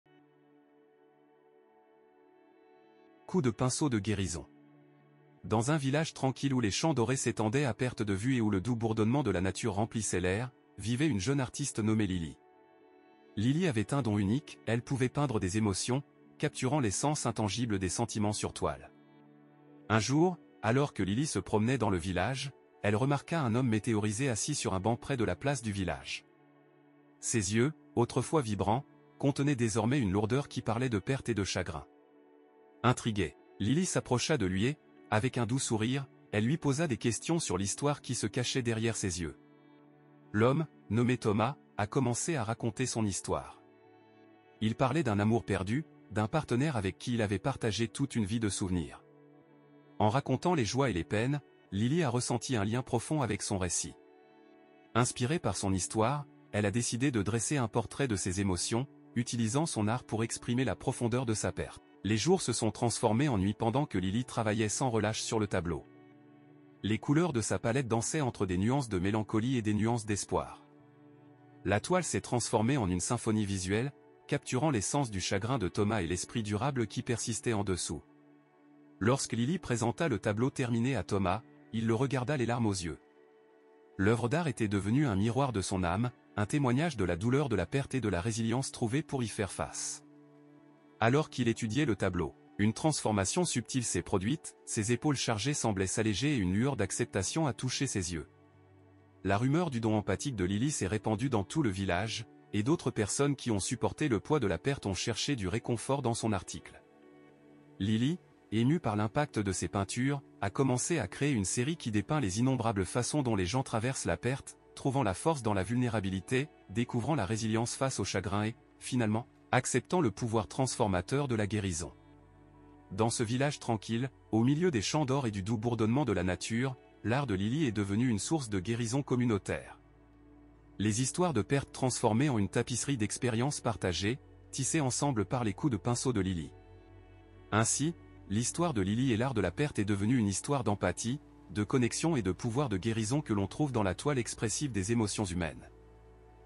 Histoires courtes ChatGPT : coups de pinceau pour guérir l'espagnol
"Coups de pinceau de guérison"
Dans un village tranquille où les champs dorés s'étendaient à perte de vue et où le doux bourdonnement de la nature remplissait l'air, vivait une jeune artiste nommée Lily. Lily avait un don unique : elle pouvait peindre des émotions, capturant l'essence intangible des sentiments sur toile.

Un jour, alors que Lily se promenait dans le village, elle remarqua un homme météorisé assis sur un banc près de la place du village. Ses yeux, autrefois vibrants, contenaient désormais une lourdeur qui parlait de perte et de chagrin. Intriguée, Lily s'approcha de lui et, avec un doux sourire, elle lui posa des questions sur l'histoire qui se cachait derrière ses yeux.

L'homme, nommé Thomas, a commencé à raconter son histoire. Il parlait d'un amour perdu, d'un partenaire avec qui il avait partagé toute une vie de souvenirs. En racontant les joies et les peines, Lily a ressenti un lien profond avec son récit. Inspirée par son histoire, elle a décidé de dresser un portrait de ses émotions, utilisant son art pour exprimer la profondeur de sa perte.

Les jours se sont transformés en nuits pendant que Lily travaillait sans relâche sur le tableau. Les couleurs de sa palette dansaient entre des nuances de mélancolie et des nuances d'espoir. La toile s'est transformée en une symphonie visuelle, capturant l'essence du chagrin de Thomas et l'esprit durable qui persistait en dessous.

Lorsque Lily présenta le tableau terminé à Thomas, il le regarda les larmes aux yeux. L’œuvre d’art était devenue un miroir de son âme, un témoignage de la douleur de la perte et de la résilience trouvée pour y faire face. Alors qu'il étudiait le tableau, une transformation subtile s'est produite : ses épaules chargées semblaient s'alléger et une lueur d'acceptation a touché ses yeux.

La rumeur du don empathique de Lily s'est répandue dans tout le village, et d'autres personnes qui ont supporté le poids de la perte ont cherché du réconfort dans son art. Lily, émue par l'impact de ses peintures, a commencé à créer une série qui dépeint les innombrables façons dont les gens traversent la perte : trouvant la force dans la vulnérabilité, découvrant la résilience face au chagrin et, finalement, acceptant le pouvoir transformateur de la guérison.

Dans ce village tranquille, au milieu des champs d'or et du doux bourdonnement de la nature, l'art de Lily est devenu une source de guérison communautaire. Les histoires de perte transformées en une tapisserie d'expériences partagées, tissées ensemble par les coups de pinceau de Lily.

Ainsi, l’histoire de Lily et l’art de la perte est devenue une histoire d’empathie, de connexion et de pouvoir de guérison que l’on trouve dans la toile expressive des émotions humaines.